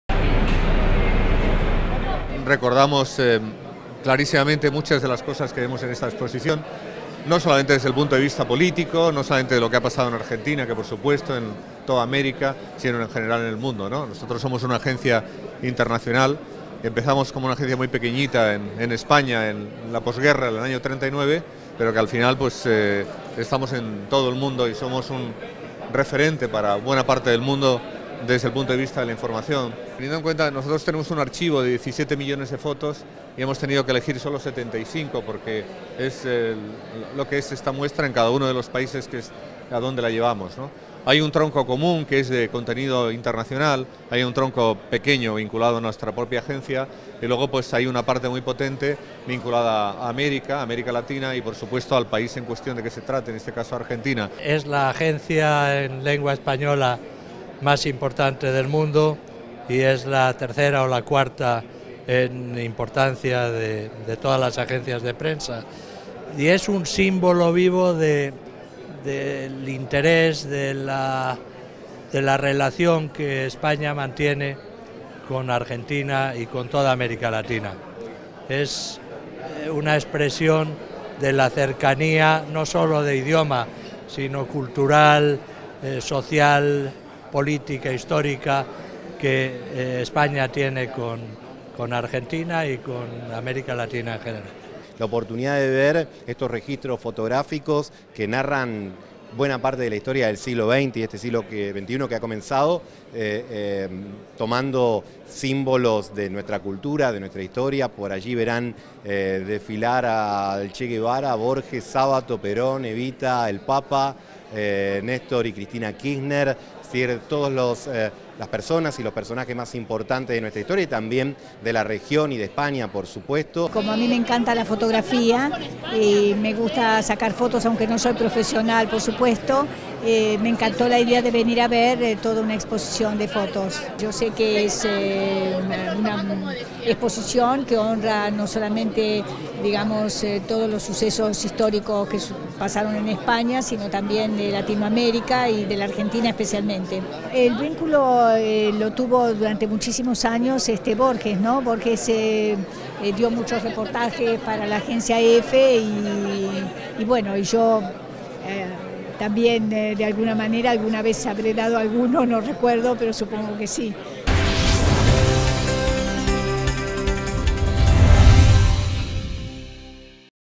EFE celebró su 75 aniversario con una exposición fotográfica
La agencia internacional de noticias en habla hispana EFE celebró en la Argentina su 75 aniversario con un particular homenaje a América Latina: una exposición en el Museo del Banco Provincia que repasa casi un siglo de historia en fotografías.

El presidente de EFE, José Antonio Vera, explicó a Télam que la muestra “es un repaso a lo que ha sido nuestra vida en este tiempo” y señaló que “empezamos como una agencia muy pequeña en España, en la posguerra, en el año 39, pero hoy estamos en todo el mundo y somos un referente desde el punto de vista de la información”.

Por su parte, el presidente del Banco Provincia, Claudio Marangoni, calificó a la exposición como “una muestra del arte gráfico que refleja con una mirada periodística parte de la historia” y donde “desfilan el 'Che' Guevara, Borges, Sabato, Perón, Evita, el Papa, Néstor y Crisitina Kirchner”, entre otras figuras importantes.

También participaron del evento el embajador de España en la Argentina, Estanislao de Grandes Pascual; María Kodama, viuda de Jorge Luis Borges, y el historietista argentino Joaquín Salvador Lavado, "Quino".